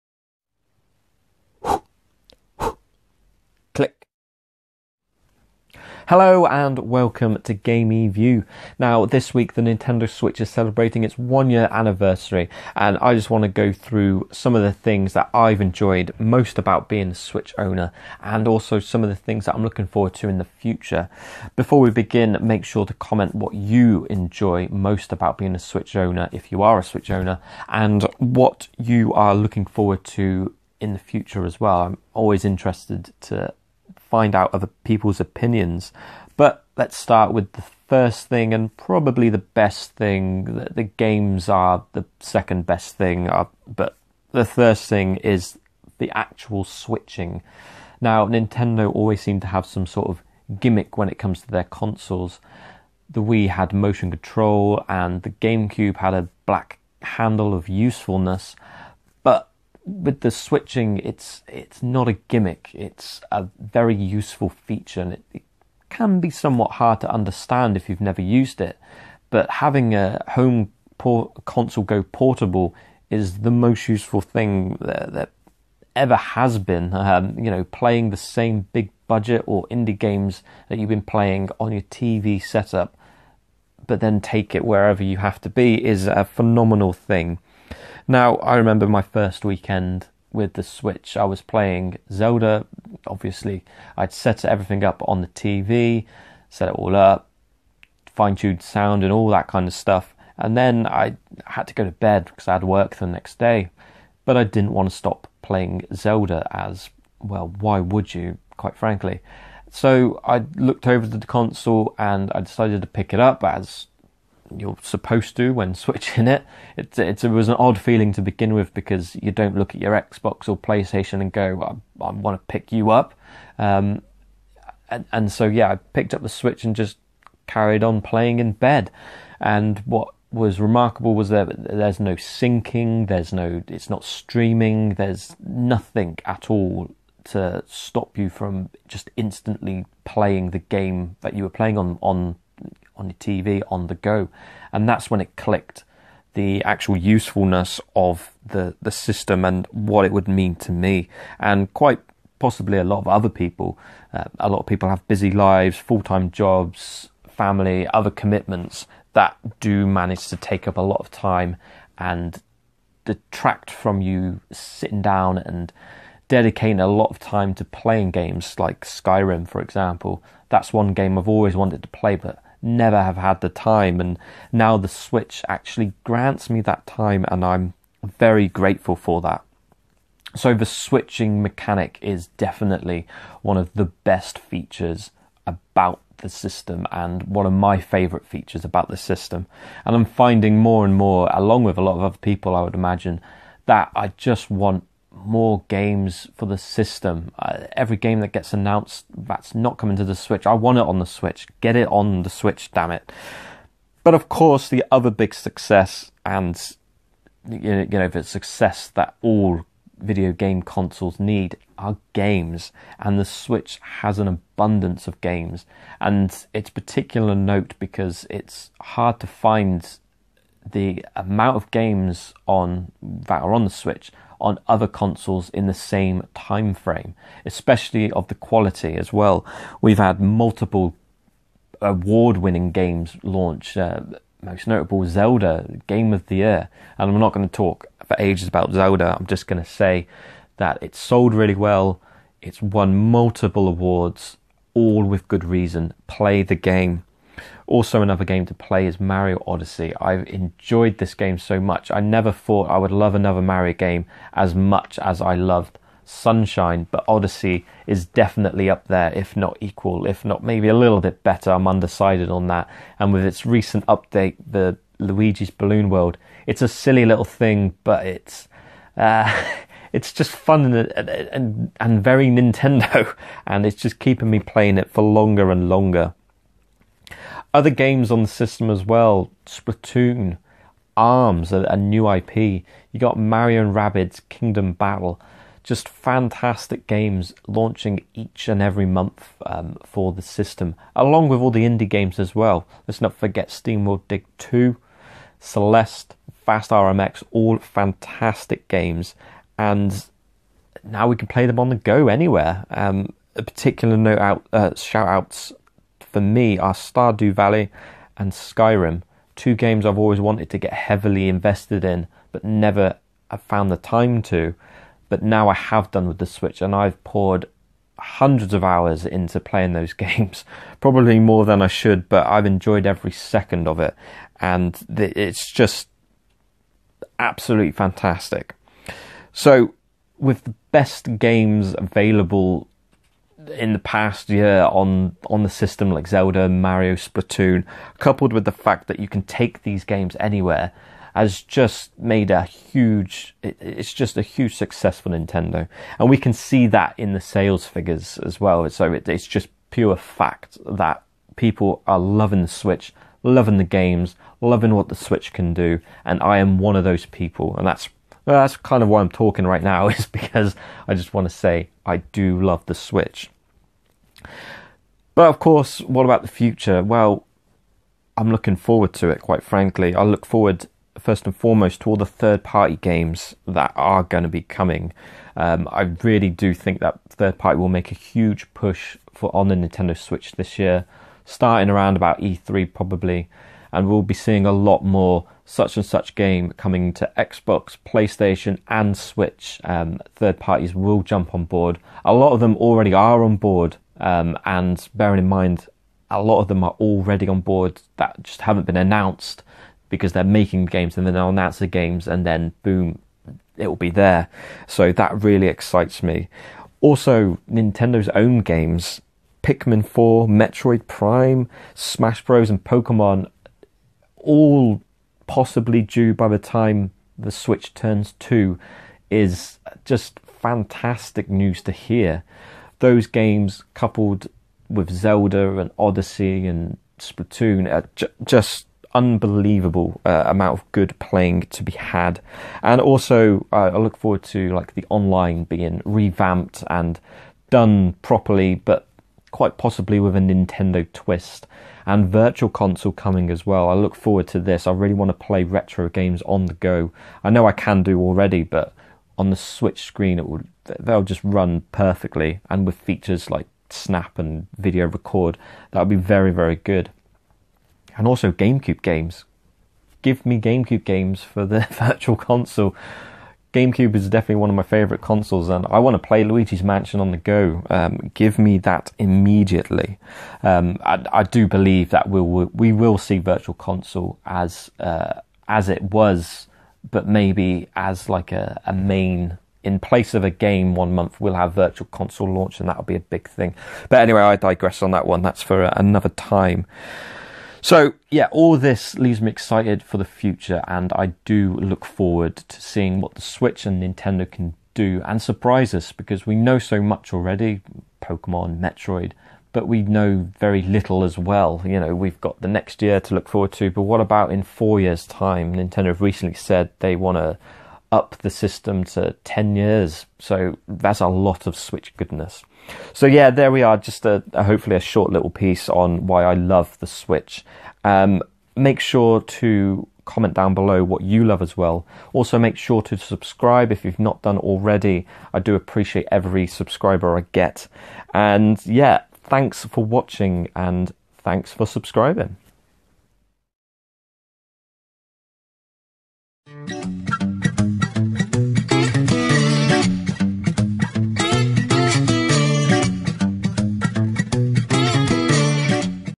Nintendo Switch - 1st Year Thoughts!
Nintendo Switch first year thoughts! the Nintendo Switch is turning 1 and celebrating its first anniversary being on the market and i thought I would give some of my impressions of what that year has brought the Switch. These are my opinions so feel free to let me know yours!